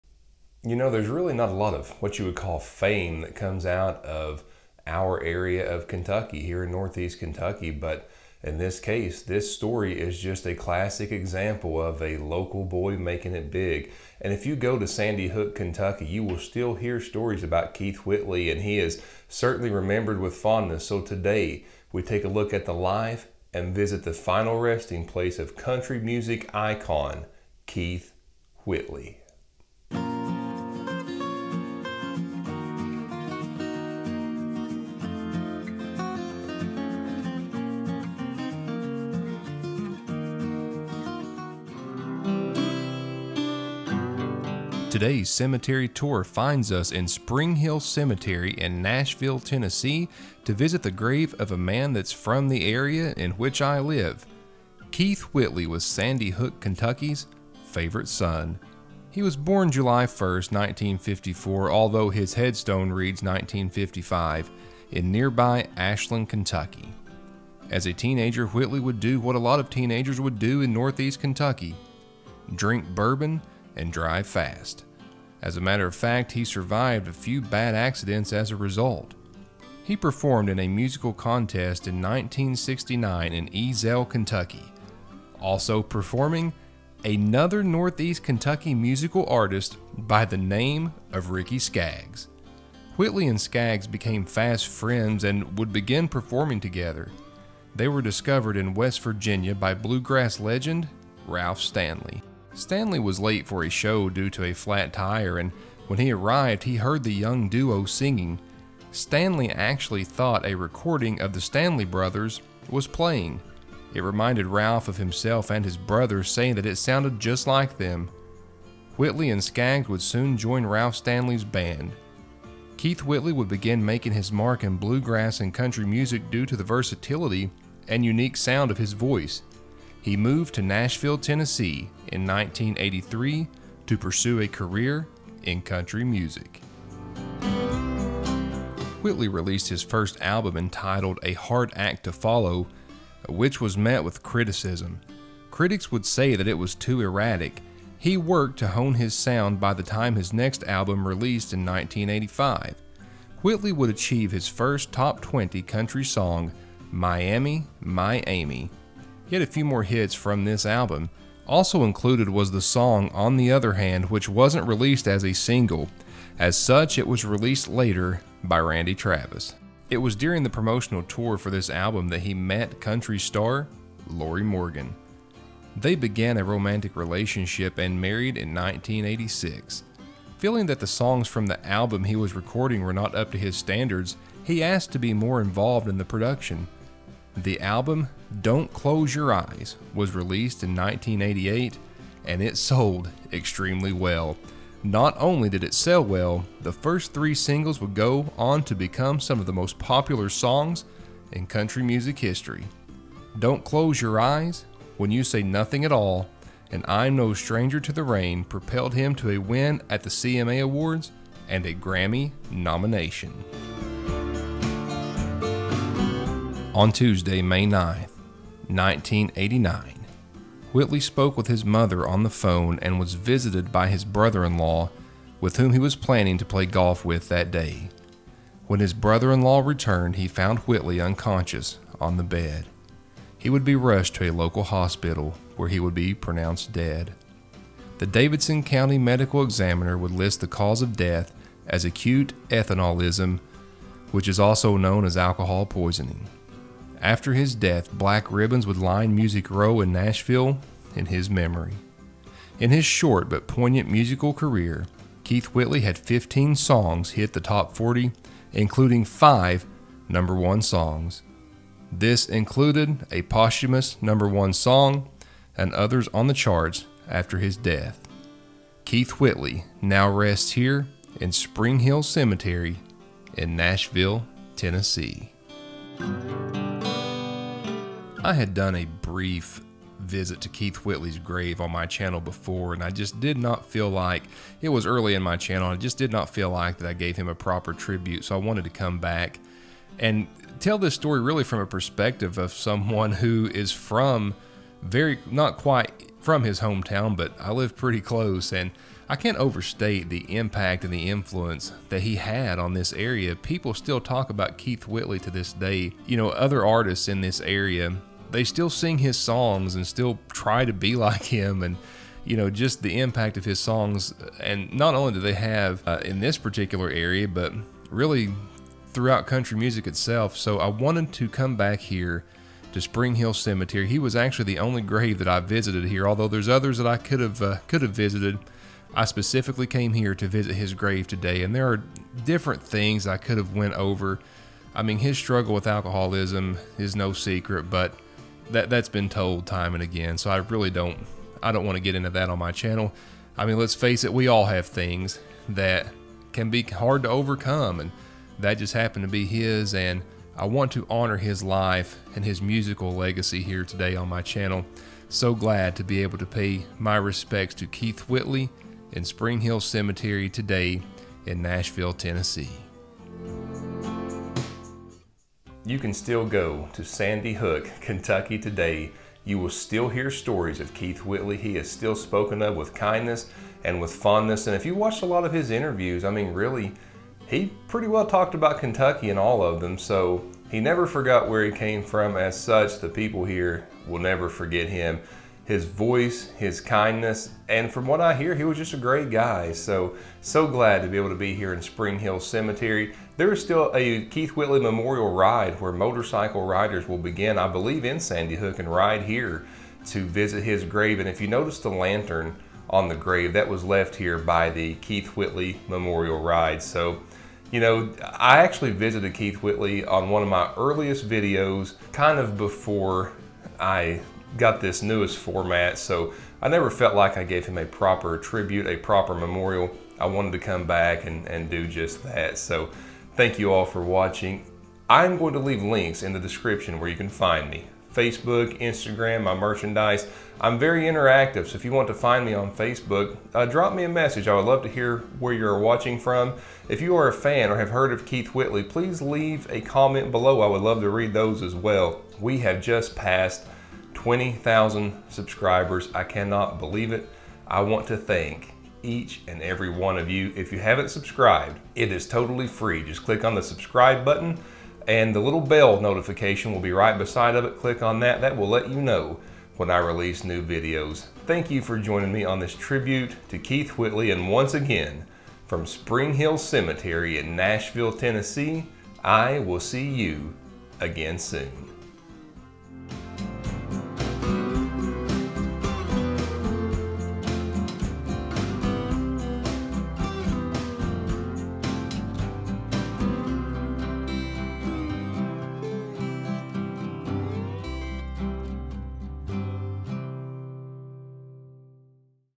Famous Graves - The Life and Gravesite of Keith Whitley
Today’s famous grave cemetery tour takes us to Spring Hill Cemetery. We are here to visit the grave of country music superstar Keith Whitley. In his short life, Whitley made many lasting contributions.

Please visit me! I would love to hear from you.